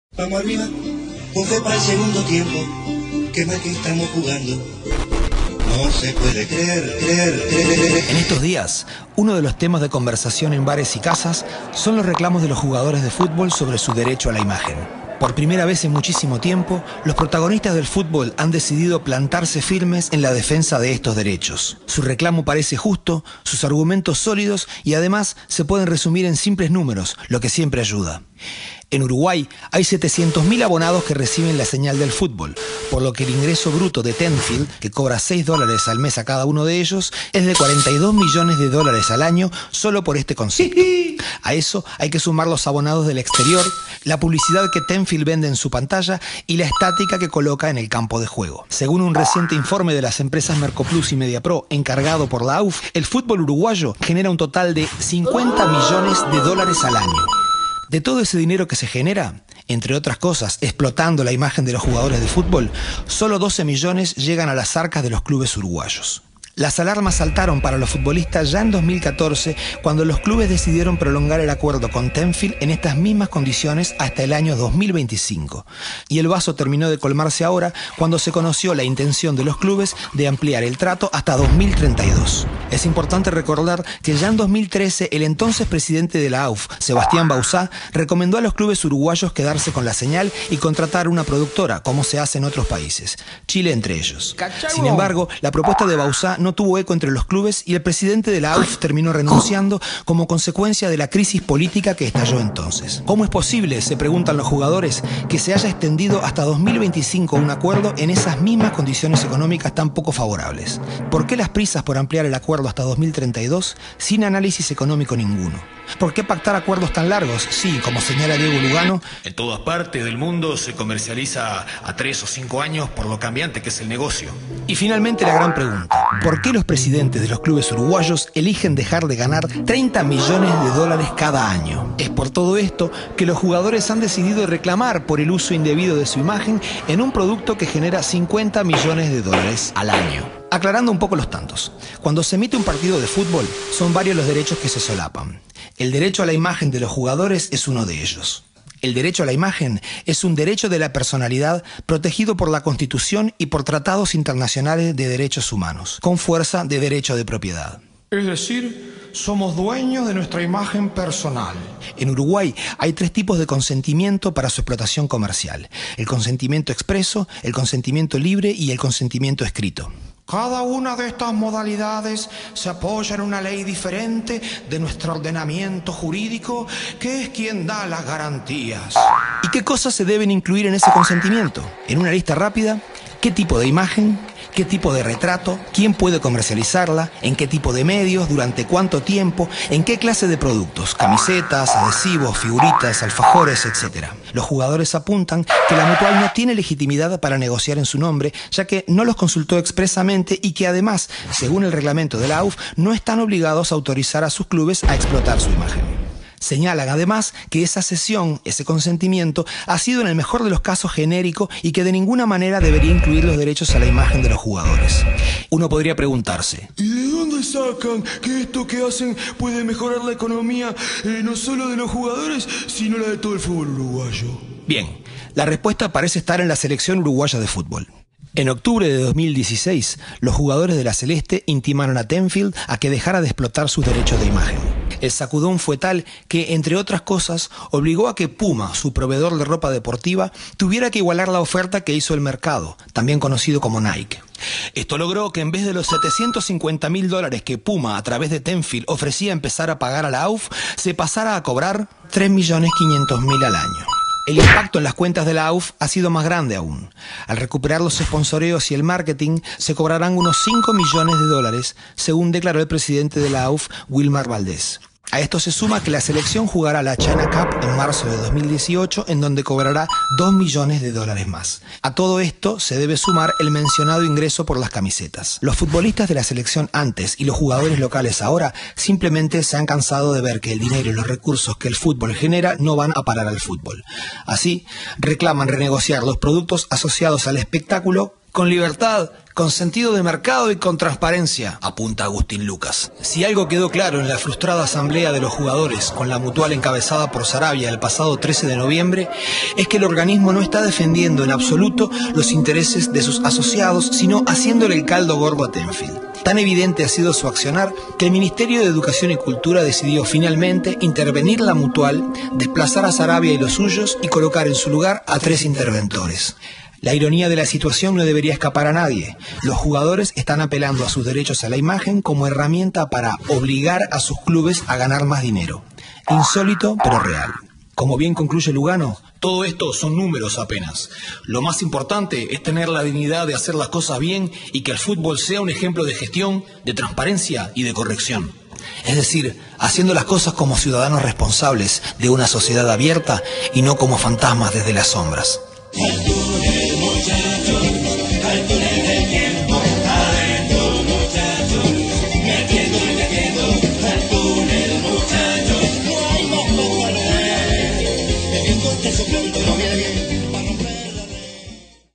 Los derechos de imagen en un video
TirandoParedes accedió a un video el cuál explica de forma lúdica la defensa de los jugadores del fútbol uruguayo por sus derechos de imagen.